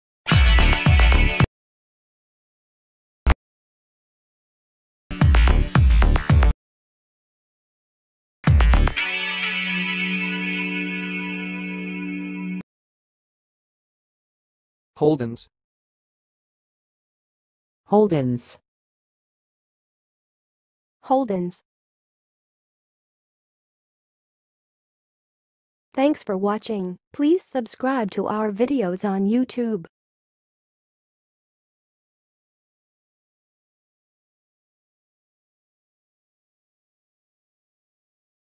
How to say "holden's"! (High Quality Voices)
Watch in this video how to say and pronounce "holden's"!